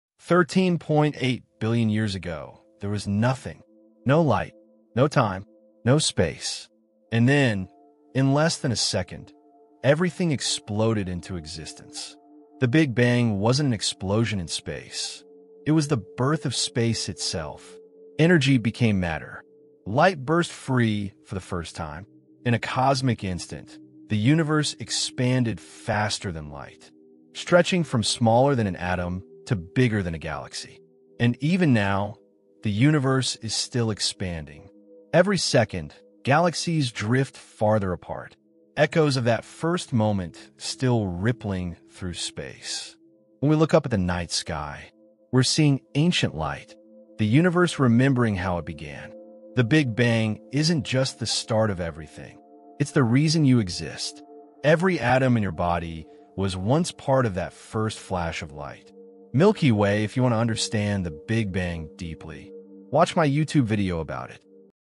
From Nothing to Everything — The Birth of the Universe
13.8 billion years ago, there was nothing.
Then — in less than a second — everything burst into existence.

This is the story of the Big Bang, the moment space and time were born.
Learn how the universe expanded faster than light, created the first stars, and set the stage for life itself.